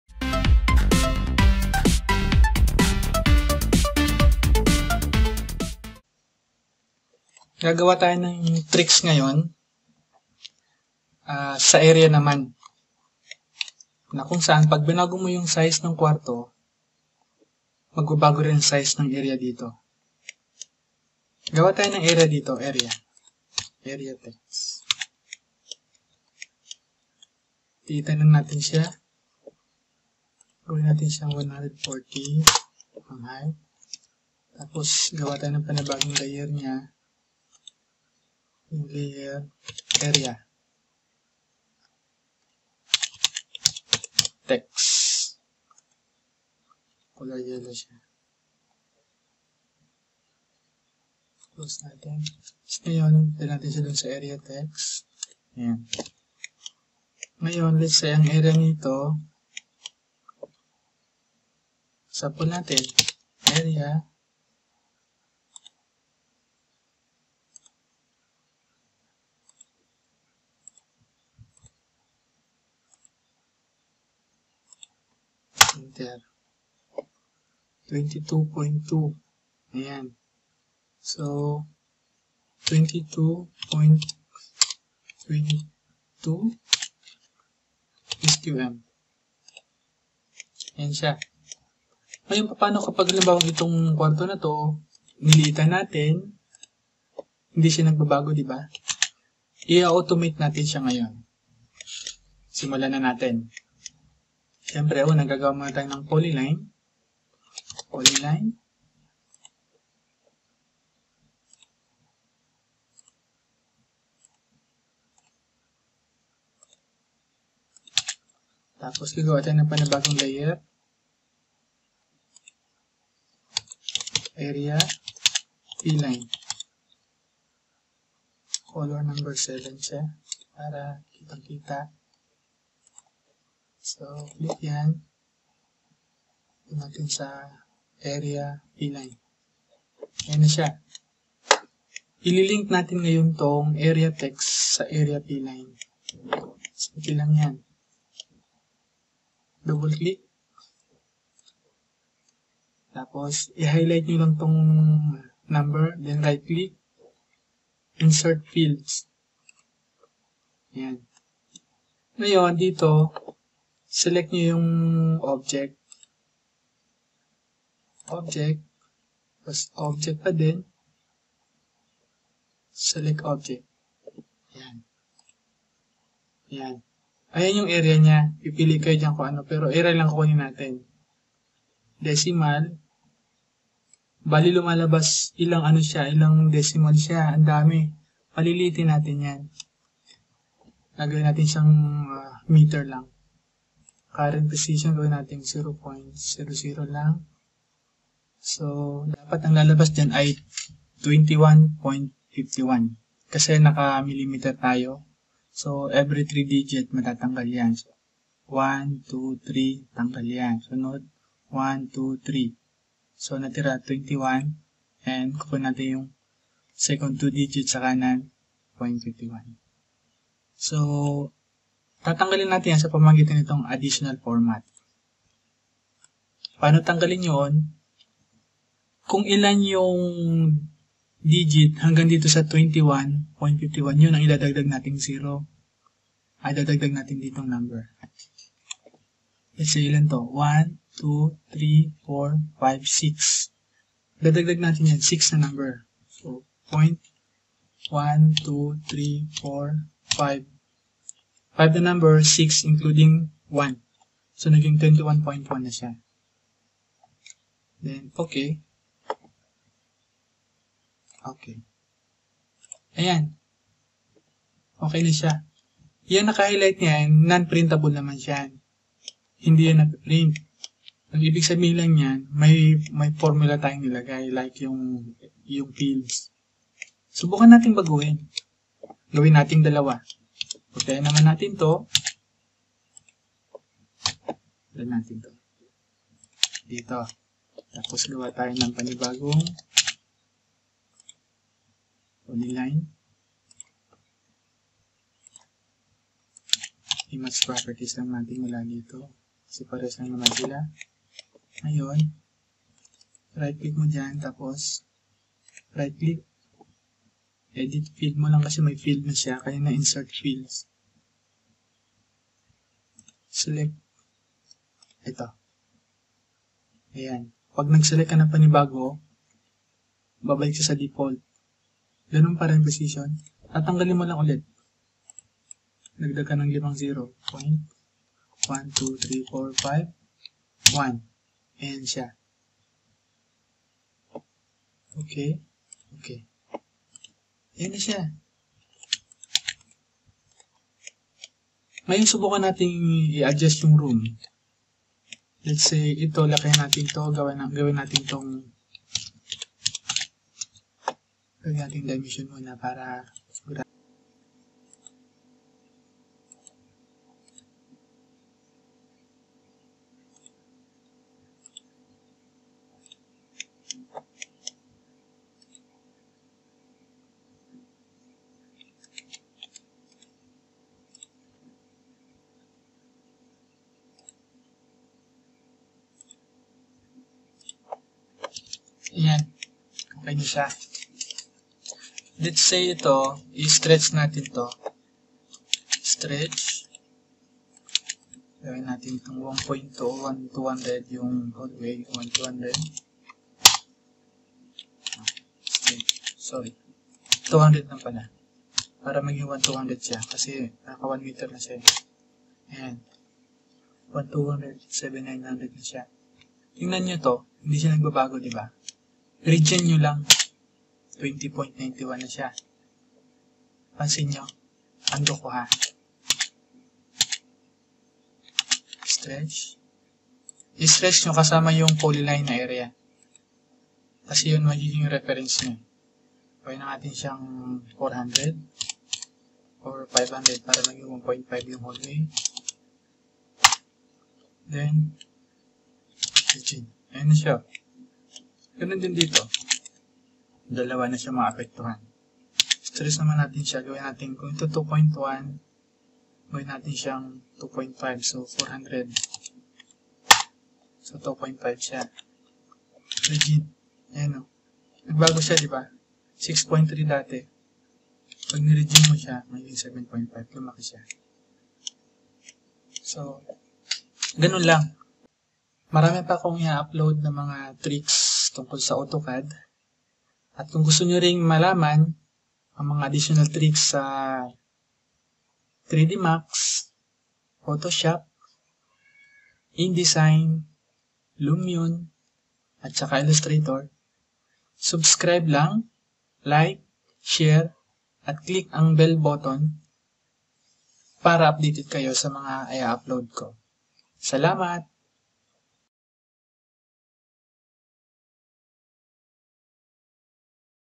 AutoCAD Tutorial | Tricks | Auto Update Room Area | Tagalog
This AutoCAD Tutorial will show you the demonstration how to link the text to Pline and automatic update the text area whenever the room size changed.

Equipment Used:
16GB Ram
2 TB

More tutorial will be uploaded soon like: 3D Studio Max, Revit Architecture, Lumion Rendering, Vray, Sketchup, Adobe Photoshop, InDesign and Illustrator